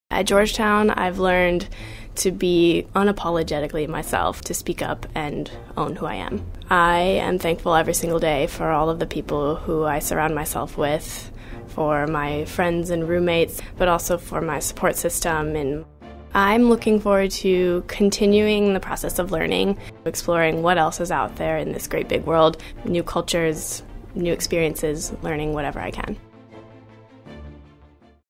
Graduating Thoughts from Caitlin (SFS'18)
What goes through a Georgetown senior's mind as they prepare to graduate from college? Caitlin from the Class of 2018 is excited to continue the process of learning, outside of the traditional classroom.